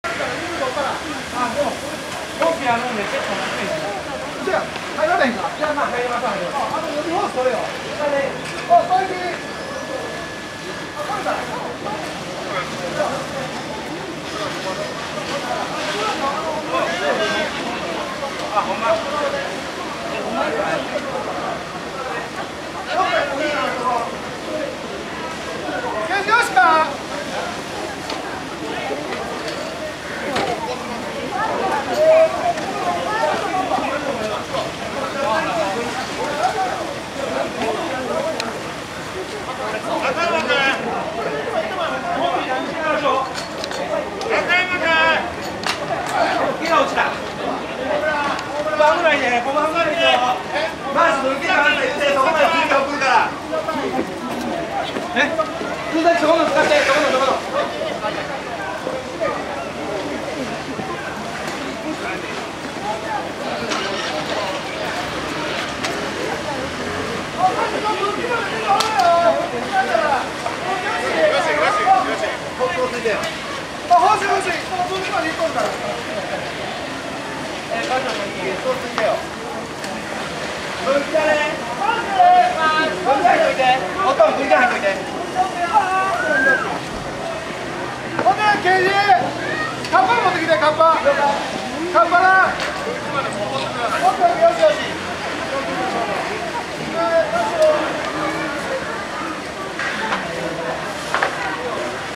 東大阪で住宅全焼、２０歳代女性が逃げ遅れ死亡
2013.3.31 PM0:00
東大阪市六万時長の民家で火災
若い女性が死亡。
まさか、この昼間に逃げ遅れるなんて・・・
お気の毒に（涙）